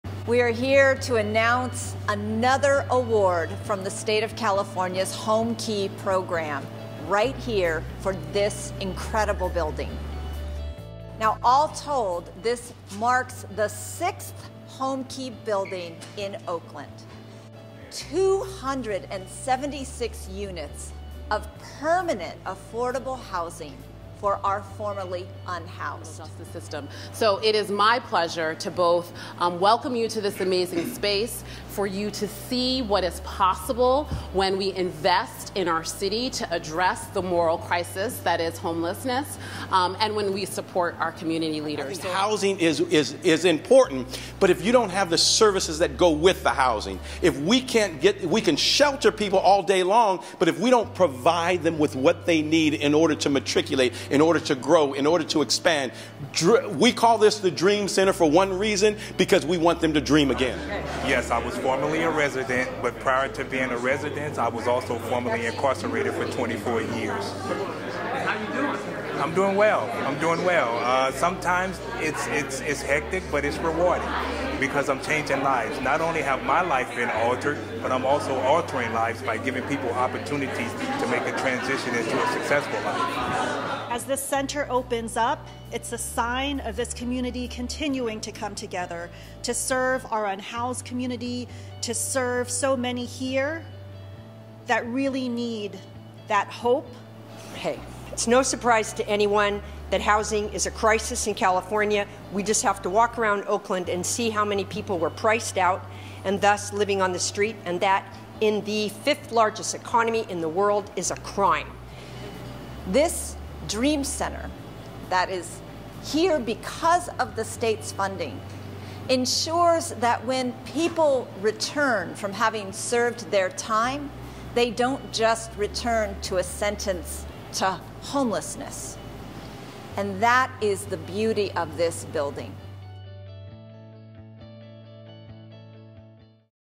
Announcing Homekey Funding for the Dream Center
Oakland Mayor Libby Schaaf, State Senator Nancy Skinner, Council President Nikki Fortunato Bas, and Housing and Community Development Director Shola Olatoye announce California Homekey funding for the Dream Center in Oakland's San Antonio neighborhood, run by Pastor LJ Jennings.